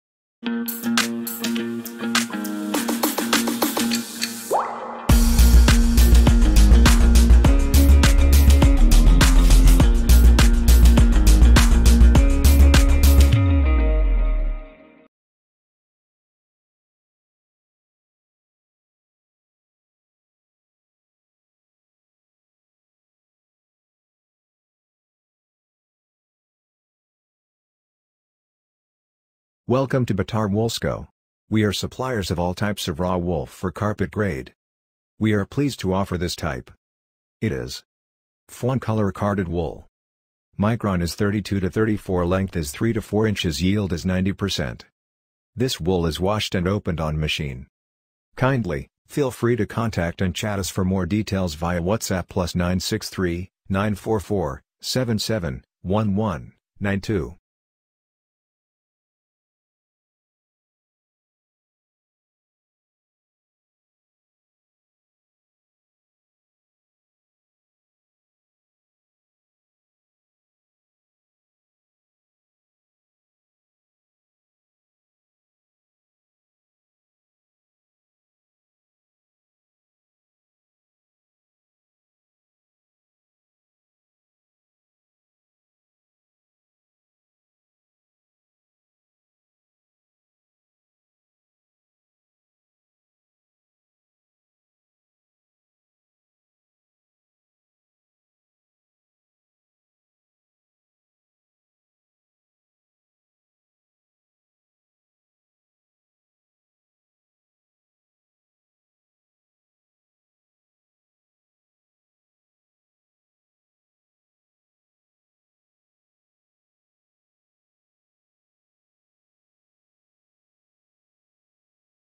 Fawn colour carded wool
Origin : Syrian wool
Micron : 32-34 Micron for carpet grade
the yield : 90% approximately
Length : 3-4 inches
the wool is washed and opened on machine

Bittarwoolsco.